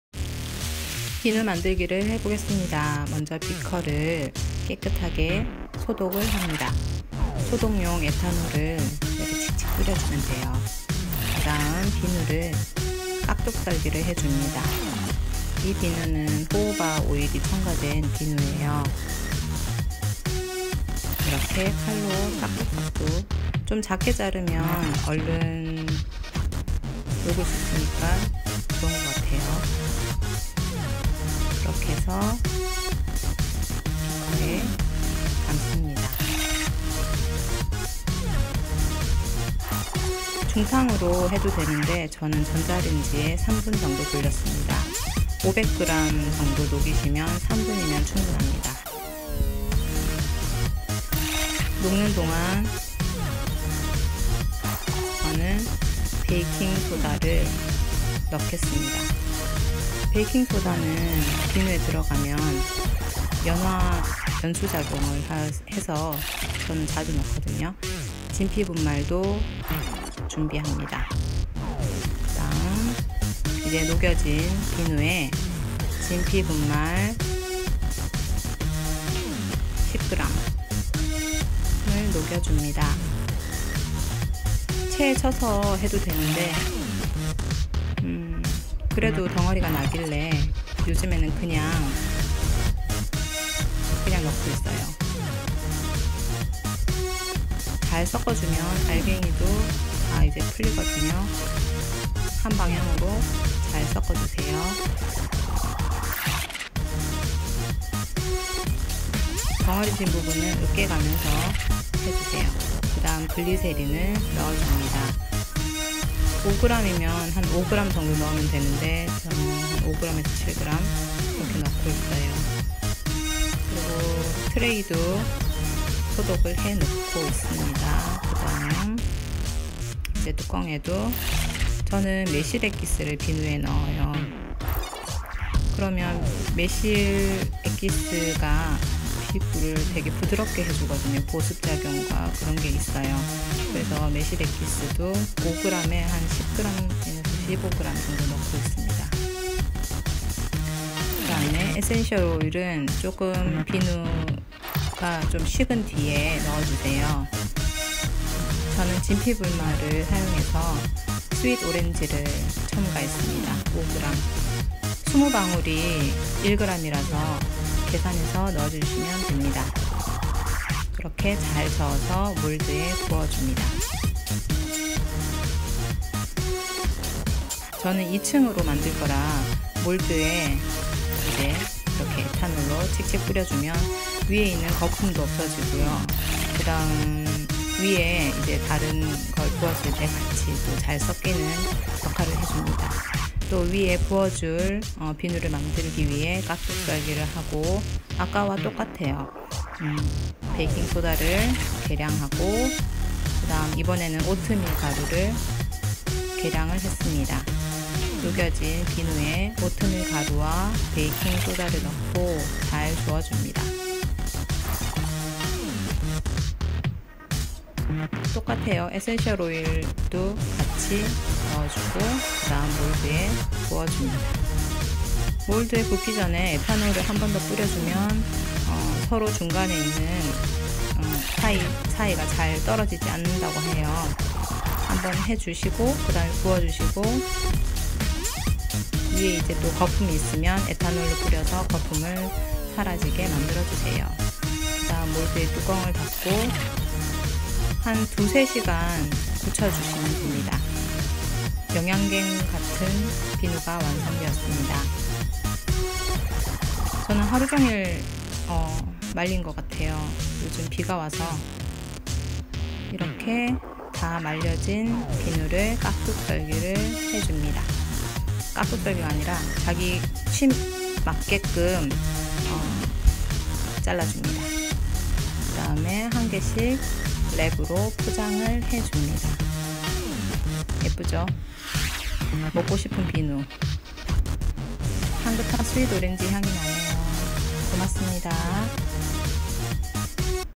진피 오트밀 천연비누만들기
비타민이 많은 진피가루와 각질제거 효과가 있는 오트밀로 비누를 만들었어요
베이스는 호호바오일이 첨가된 베이스를 사용했답니다
스윗오렌지 에센셜 오일이랑 참 잘 어울립니다